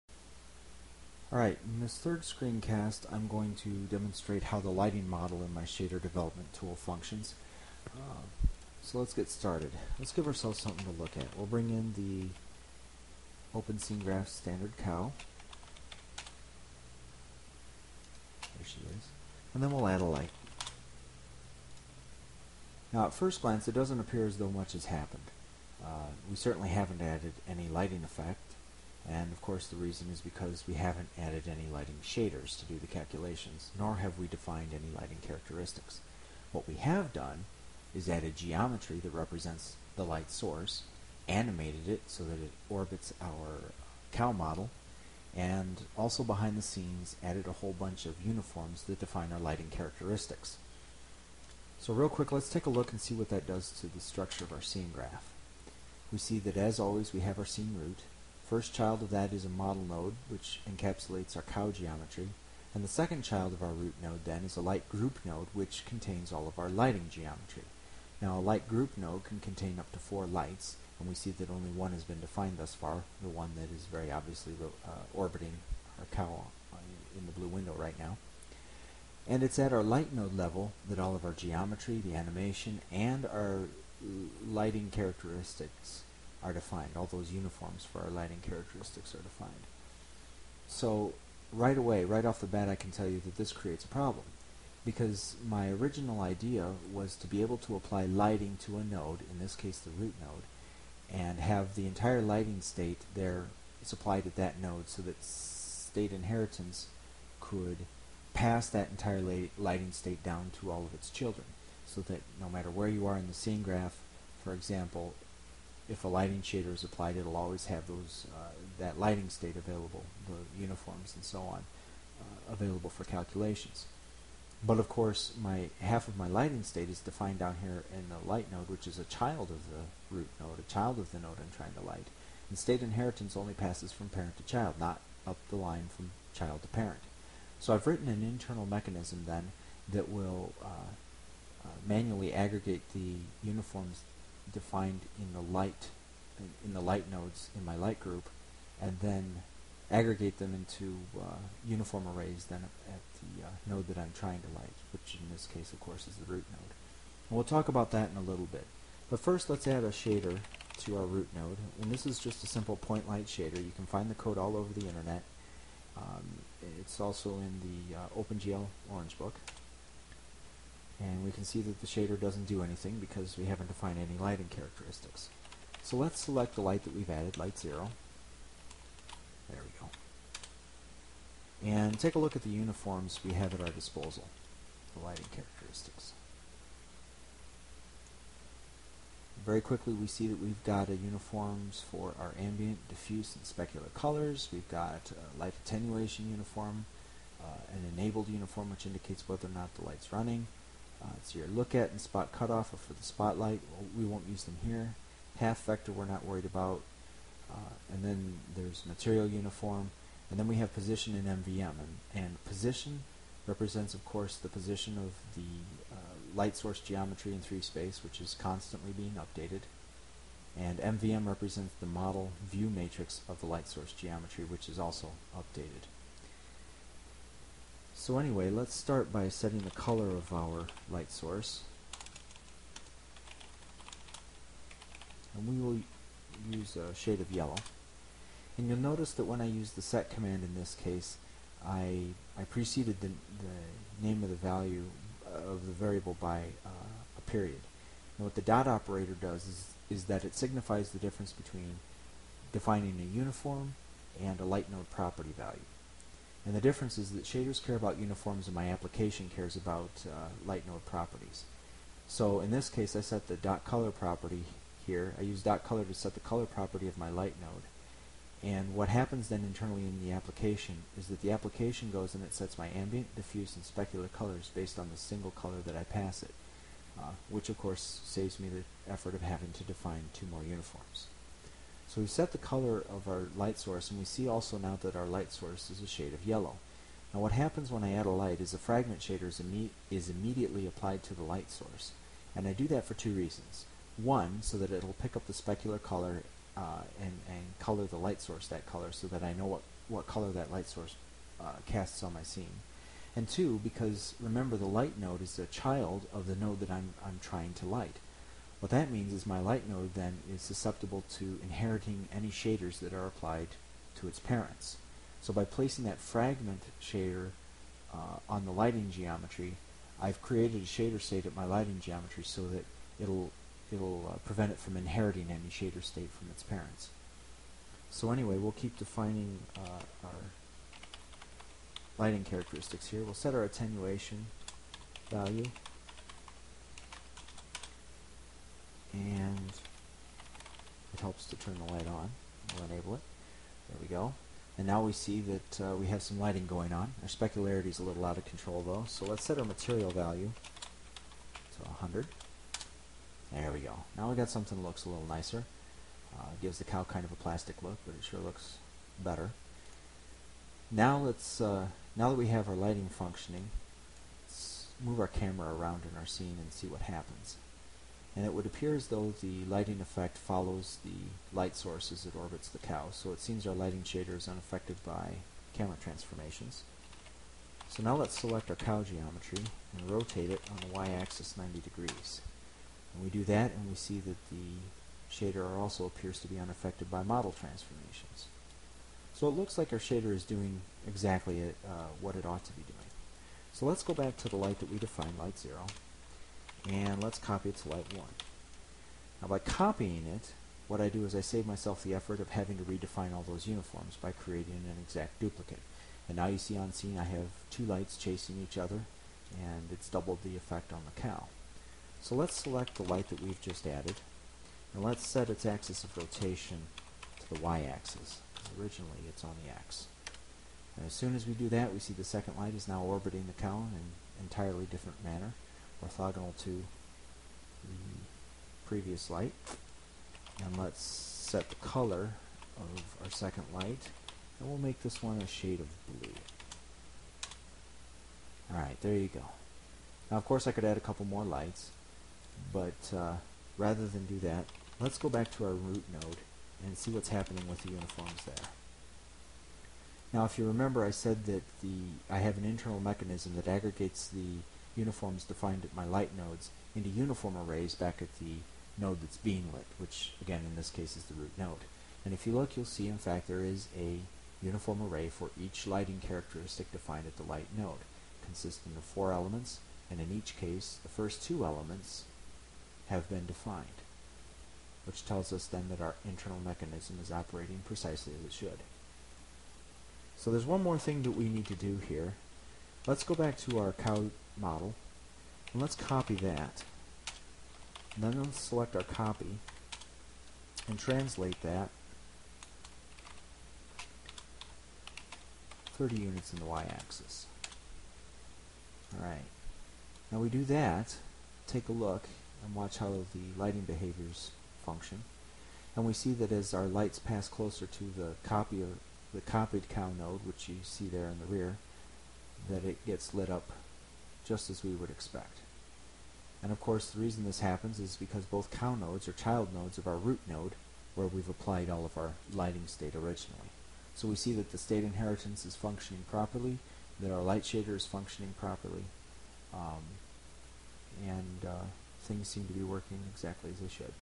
Shadow Grounds OSG #3 - Scene Lighting
Demo of scene lighting implementation for my shader dev. environment.